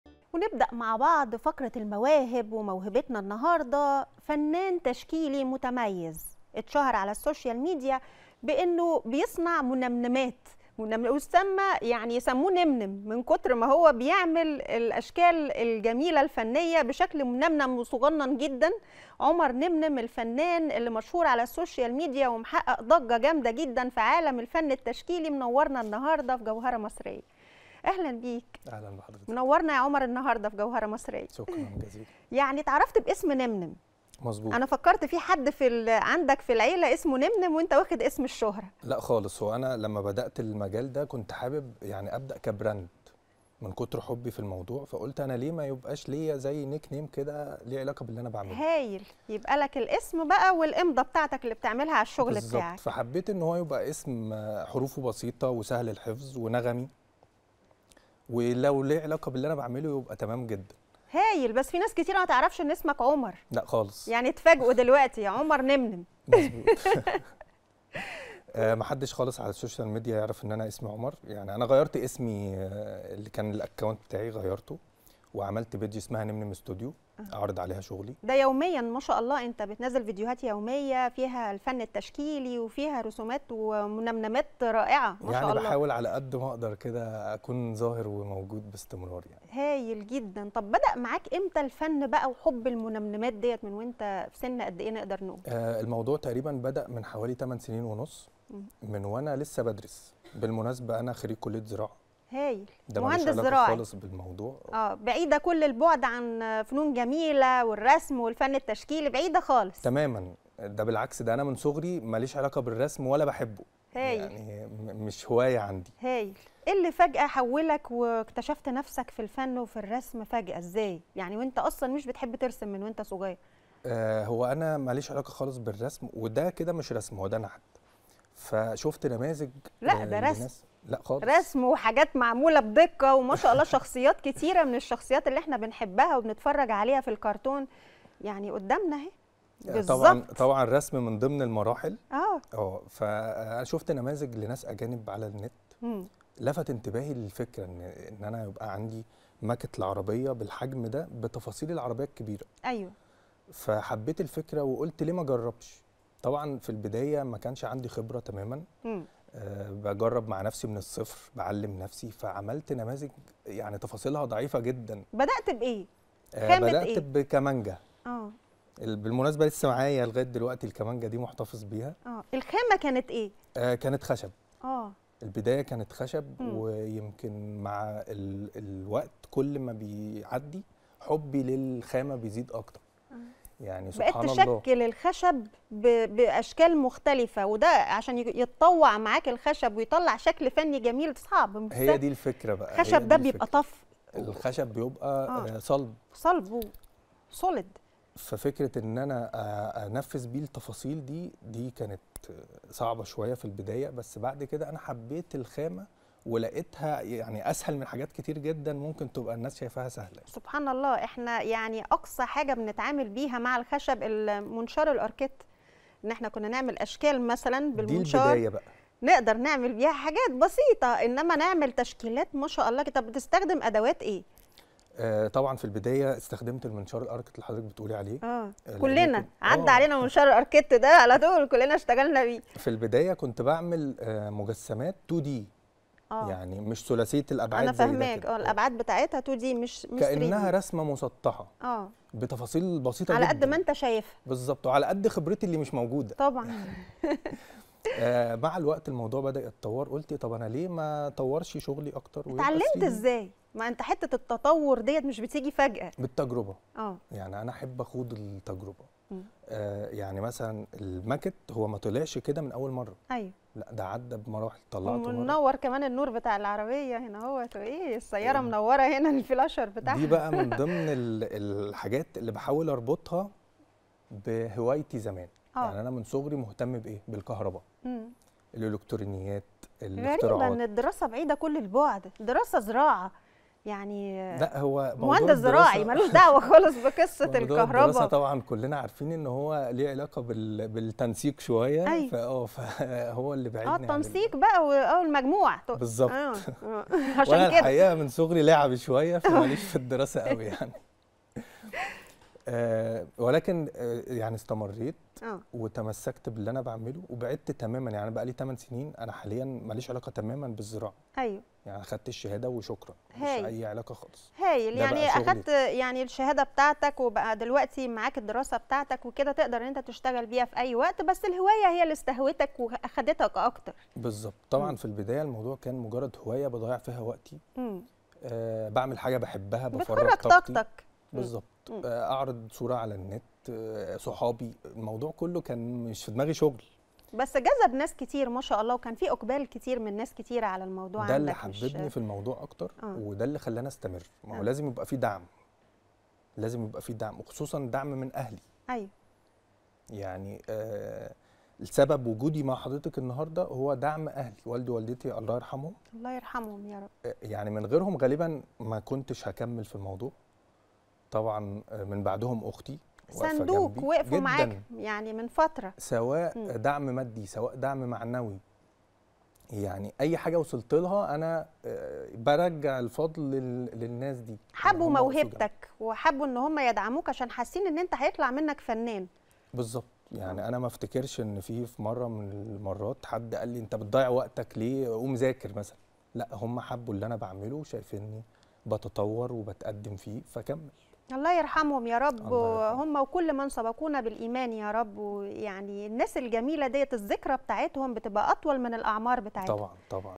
سمعت عن فن النمنمات عمر نمنم فنان تشكيلي موهوب في الفن ده وأبهرنا بشغله مع الإعلامية هبة عبد الفتاح
سمعت عن فن "النمنمات" قبل كده عمر نمنم فنان تشكيلي موهوب في الفن ده وأبهرنا بشغله مع الإعلامية